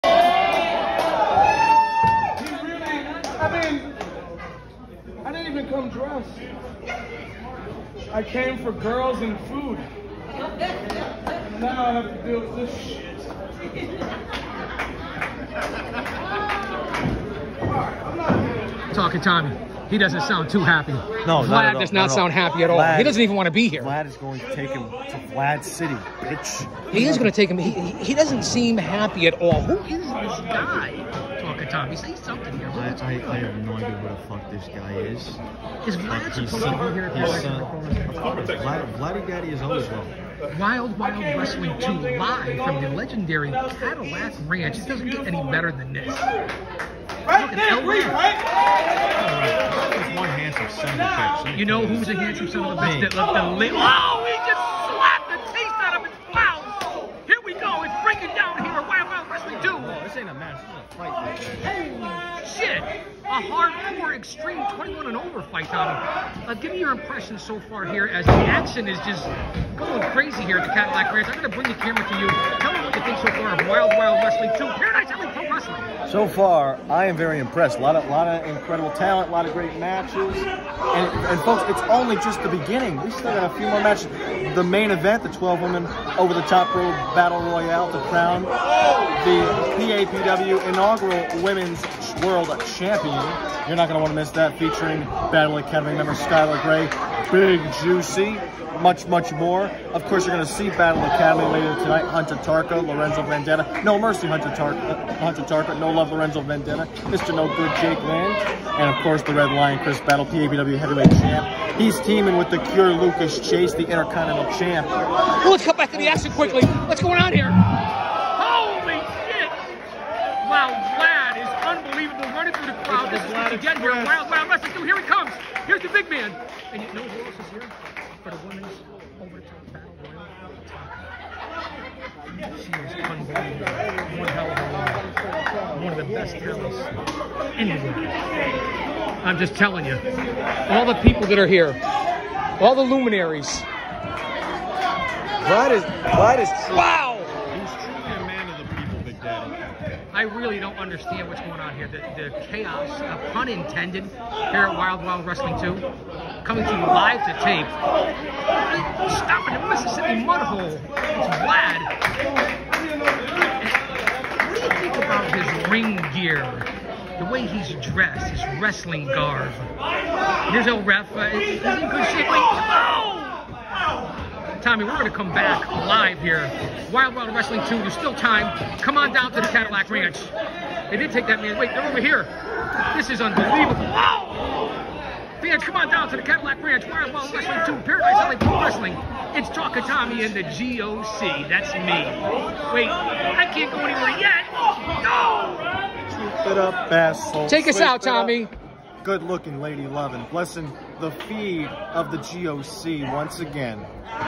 Wild, Wild, Wrestling 2(PAPW)
PAPW at the Legendary Cadillac Ranch for Wild Wild Wrestling 2! The Monster “Vlad” goes from zero to 60!!